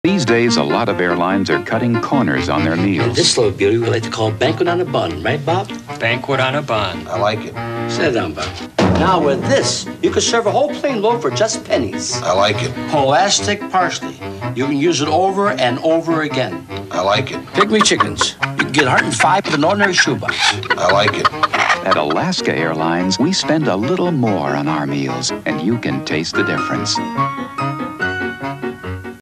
1987 Alaska Airlines Commercial
This 1987 Alaska Airlines commercial shows the ways other airlines are nickle and dimming their customers. But not Alaska. Lovely 727 at the end.